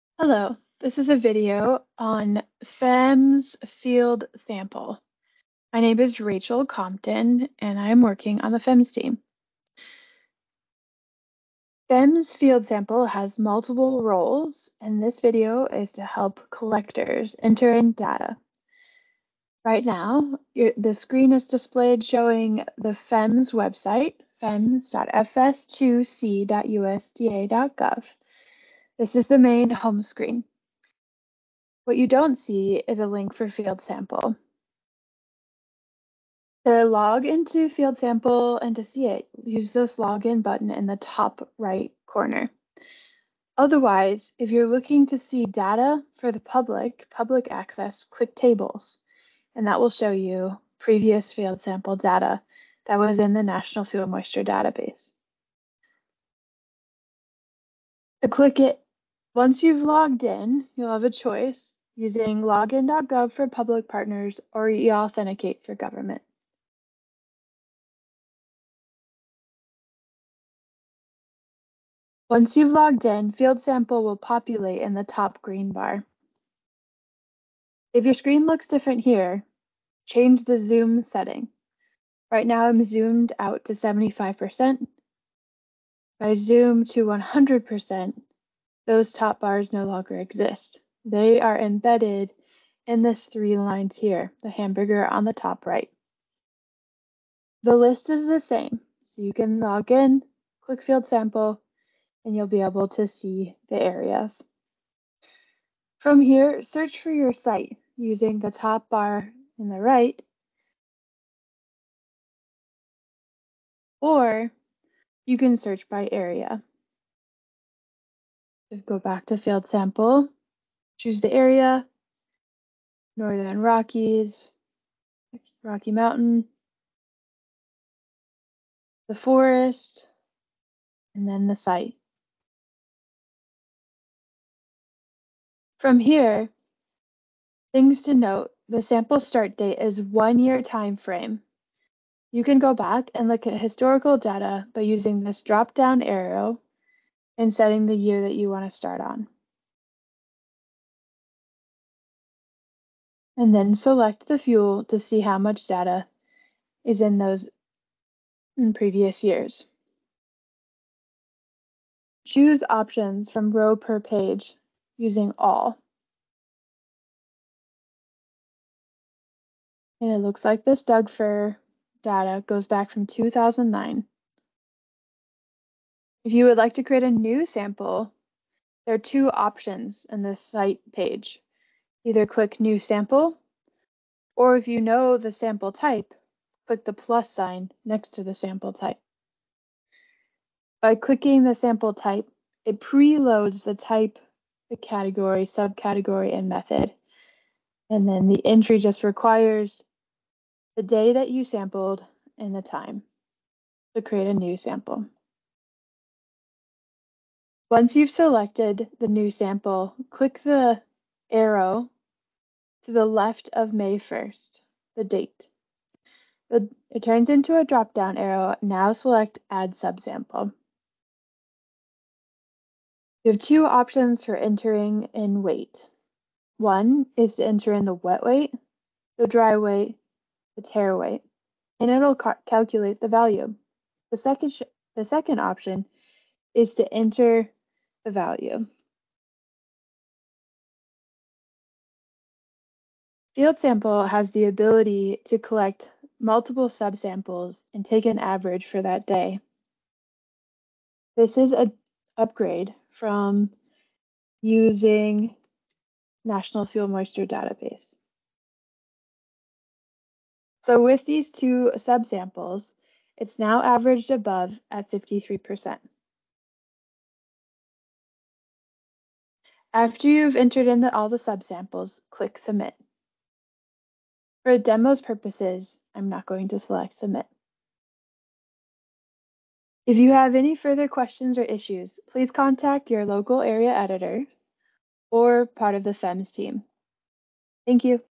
FEMS Site Collector Training
This video gives a demonstration of how to input fuel moisture samples as a site collector into the Field Sample Module of FEMS.